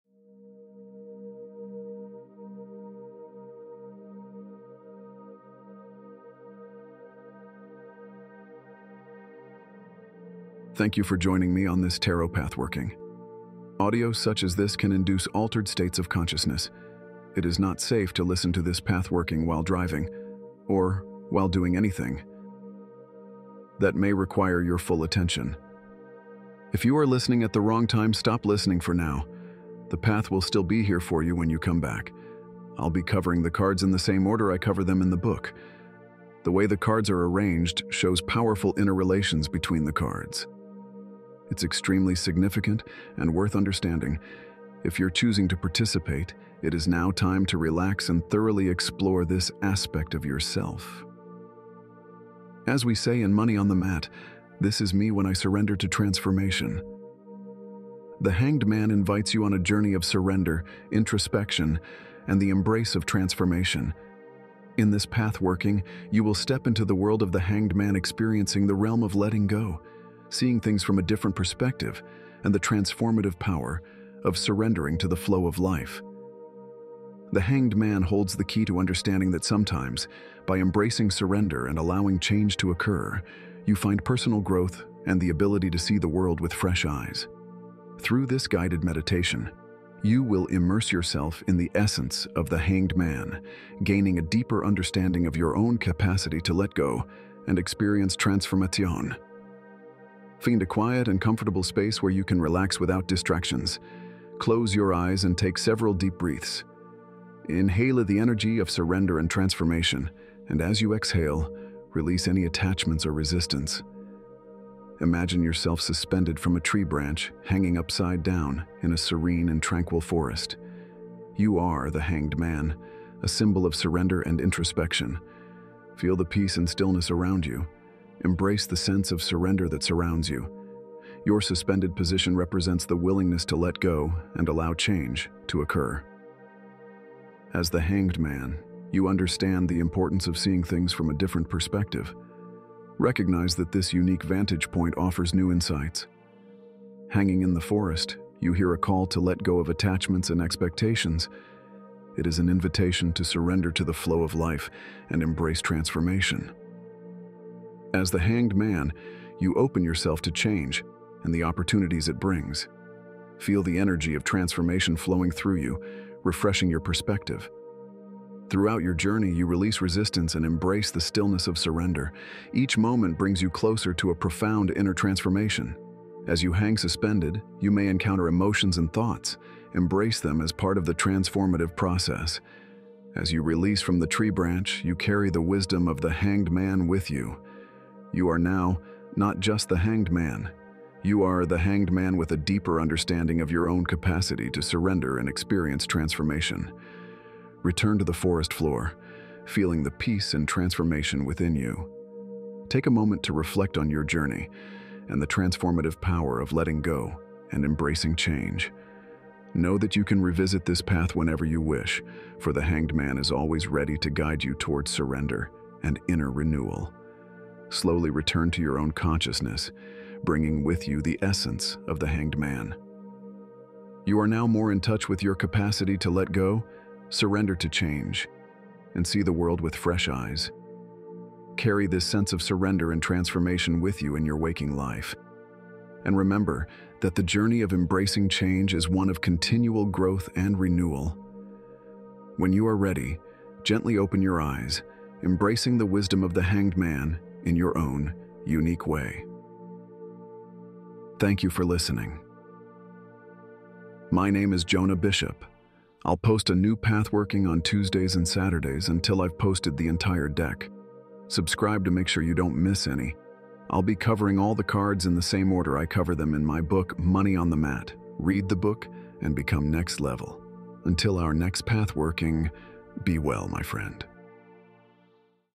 Learn The Hanged Man Tarot Card in Under Five Minutes!
Surrender to new perspectives with The Hanged Man Tarot Pathworking! In this guided meditation, you'll immerse yourself in the transformative energy of surrender and enlightenment symbolized by The Hanged Man tarot card. Explore the profound themes of release, enlightenment, and the path to embracing your inner seeker as you embark on a transformative quest for personal growth.

The Hanged Man card represents the need to let go of attachments and embrace a fresh perspective, allowing you to access the power of enlightenment and inner transformation. This pathworking invites you to step into a space of surrender, where you can release old patterns, gain insight into your inner wisdom, and discover the potential for personal growth through embracing the beauty of change and inner exploration. Through immersive experience, you'll embrace the power of surrender and the journey toward seeing the world from a new angle.

Guided by reflective narration and contemplative imagery, you'll journey through the landscapes of self-discovery and personal empowerment. Each step of this enlightening adventure will bring you closer to understanding the significance of this powerful archetype in your life. Prepare to gain clarity, release resistance to change, and experience the beauty of inner enlightenment and transformation.

Whether you're new to tarot or an experienced seeker, The Hanged Man Tarot Pathworking offers a profound and enlightening encounter. Allow the energy of surrender and enlightenment to guide you as you embark on a transformative voyage of embracing your inner seeker, gaining clarity, and embracing the power of inner transformation.